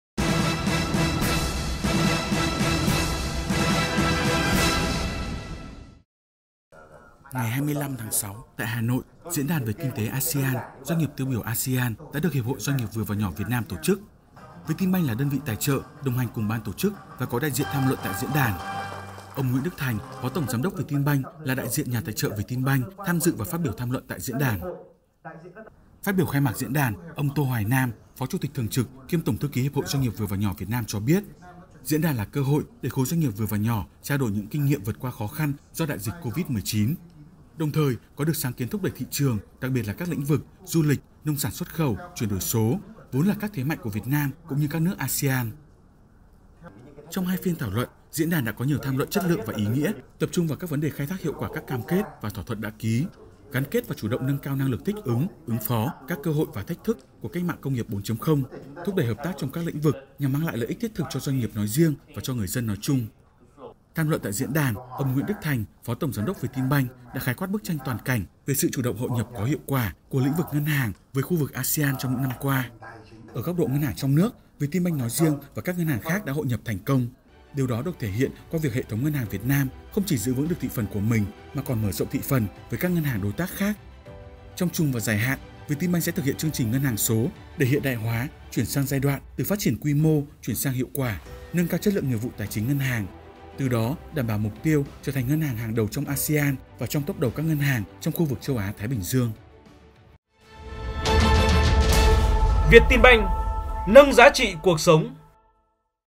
VietinBank tham dự Diễn đàn về Kinh tế ASEAN, Doanh nghiệp tiêu biểu ASEAN
Ngày 25/6/2020 tại Hà Nội, Diễn đàn về Kinh tế ASEAN, Doanh nghiệp tiêu biểu ASEAN (Diễn đàn) đã được Hiệp hội Doanh nghiệp nhỏ và vừa (DNNVV) Việt Nam tổ chức. VietinBank là đơn vị tài trợ đồng hành cùng Ban Tổ chức và có đại diện tham luận tại Diễn đàn...